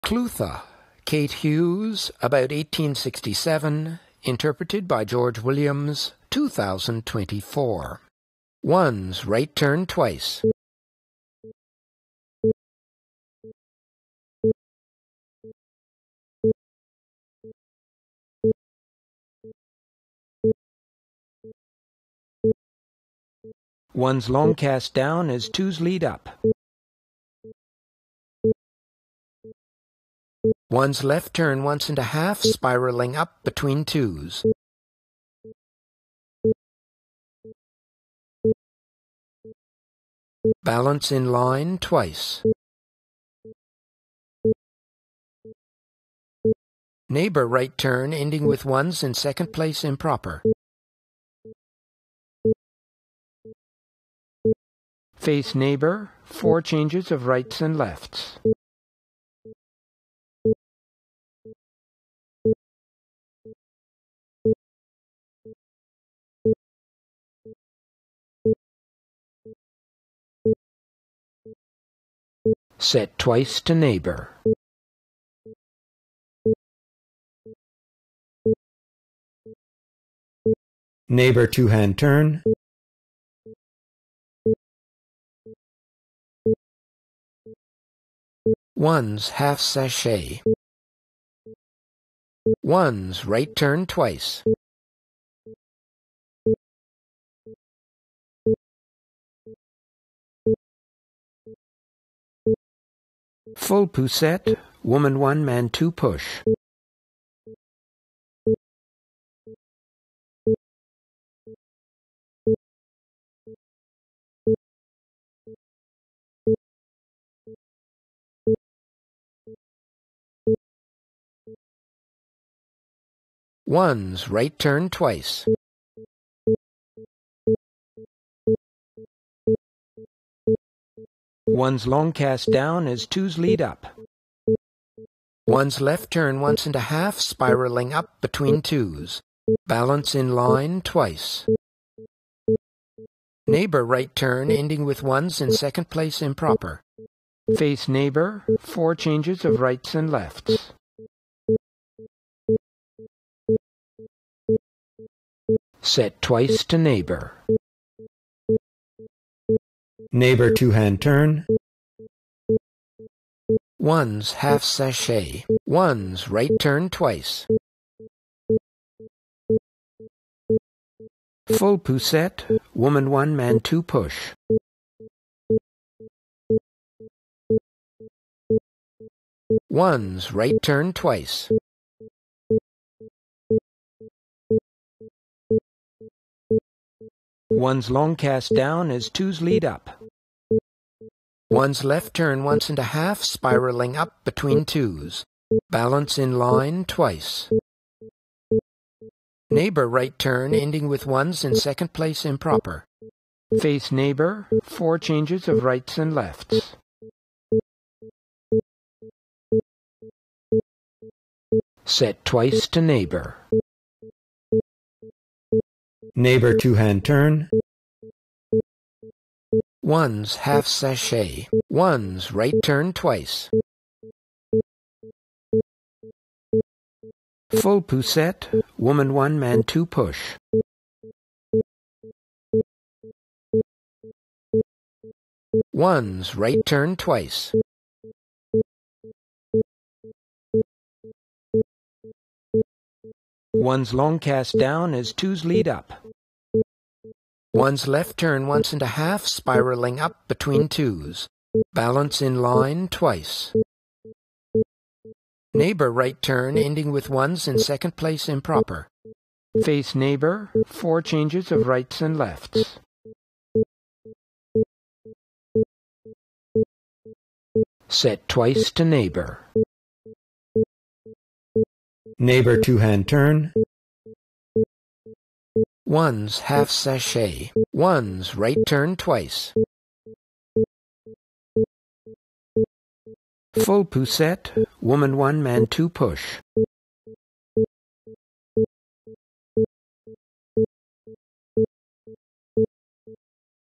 Clutha ~ Hughes
Clutha is a Country Dance written down by Kate Hughes in about 1867. It was interpreted by the George Williams. It is a proper duple minor longways dance which takes 56 bars.

Hughes wrote:
    1.Top couple swing in the centre (8)
    2. Cast a couple and swing (8)
    3. Balance four in line with second couple and turn opposite partners (8)
    4. Rights and lefts (8)
    5. Set and turn opposite partners (8)
    6. Half chasie and swing out time (8)
    7. Pousette (8)

The animation plays at 120 counts per minute normally, but the first time through the set the dance is slowed down so people can learn the figures more readily. Men are drawn as rectangles, women as ellipses. Each couple is drawn in its own color, however the border of each dancer indicates what role they currently play so the border color will change each time through the minor set.